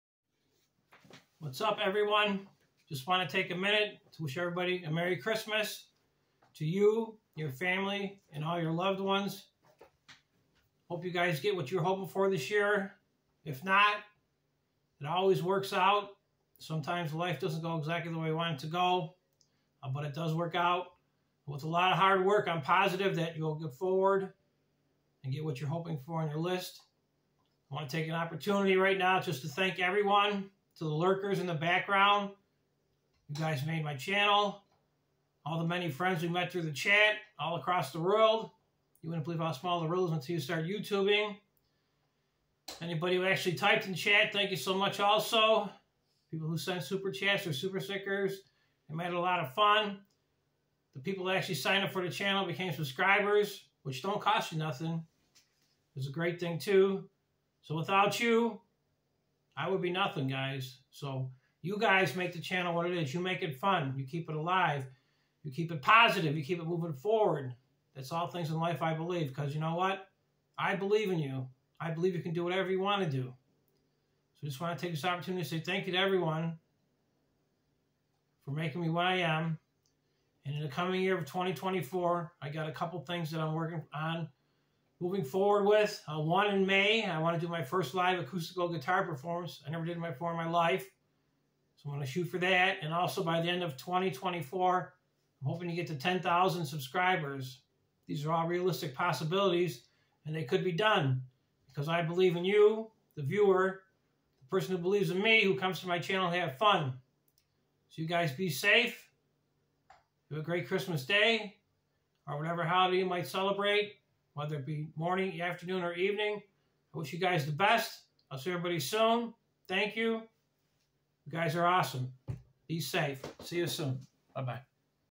Merry Christmas to all.... thank you everyone!
hello everyone its Christmas eve and i want to wish everyone of you a very Merry Christmas...hope all is well & please be safe...see everyone soon